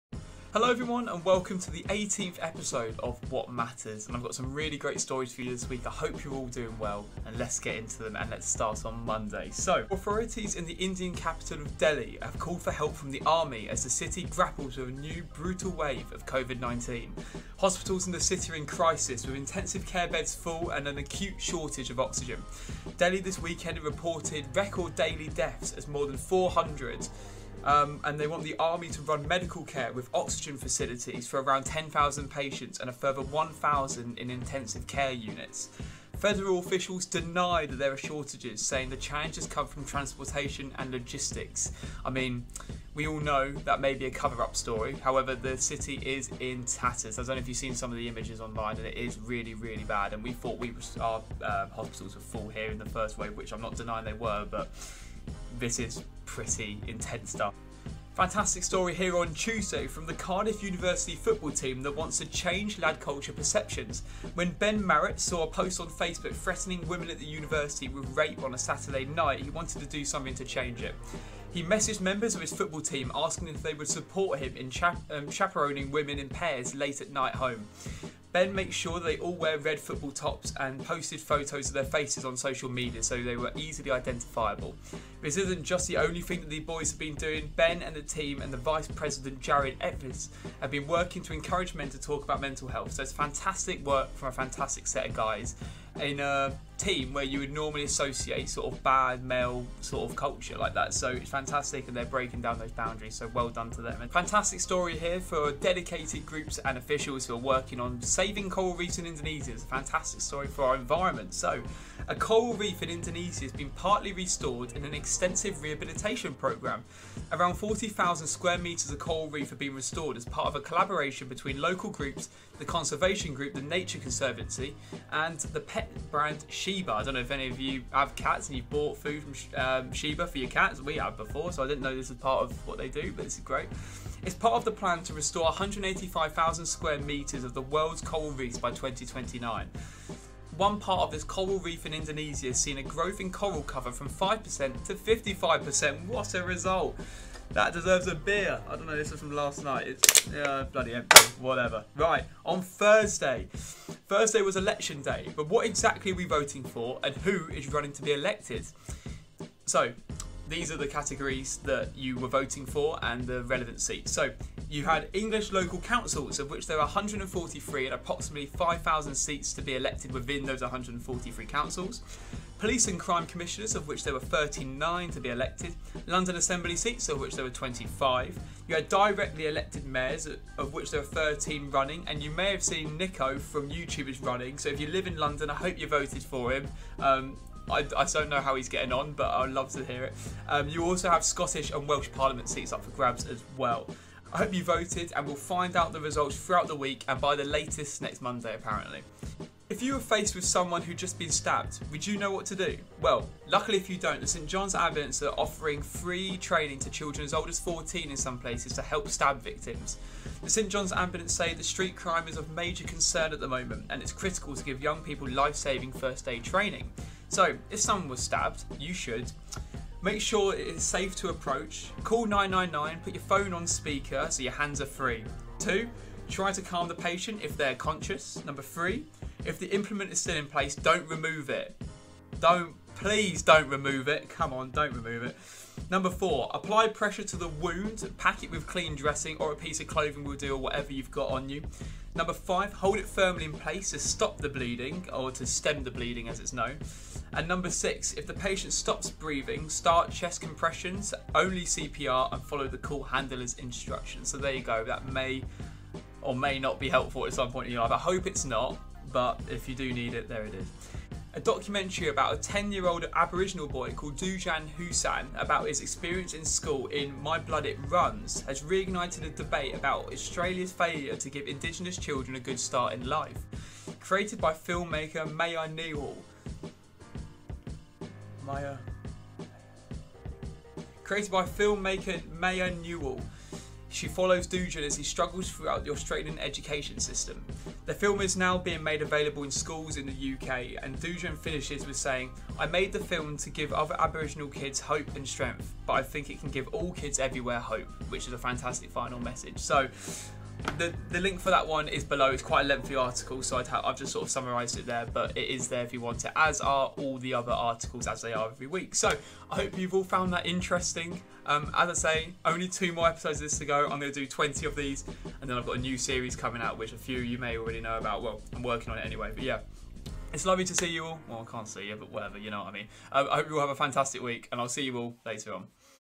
Weekly News Summaries WB 2nd May 2021!
The 18th instalment of News Summaries is upon us, and what are some of the stories that feature this week I hear you ask?

From the Election on Thursday, I run you through the seats and places up for grabs across the UK, Scotland and Wales. How one part of a coral reef in Indonesia has been restored by up to 55% thanks to a dedicated set of groups. And finally, how the Cardiff University Men's Football team are changing 'lad culture' perceptions, through some fantastic chaperoning work late at night, and talking about mental health!

Sources from this week’s summary

Monday:
India Covid pandemic: Delhi calls for army help amid crisis

Tuesday:
Cardiff University team wants to change 'lad culture' perceptions

Wednesday:
Indonesia coral reef partially restored in extensive project

Thursday:
Election 2021: How do I vote and why could results take longer?

Friday:
Knife crime: St John Ambulance to teach teens to help stab victims

Saturday:
The 'smart and cheeky' Aboriginal boy teaching Australia a lesson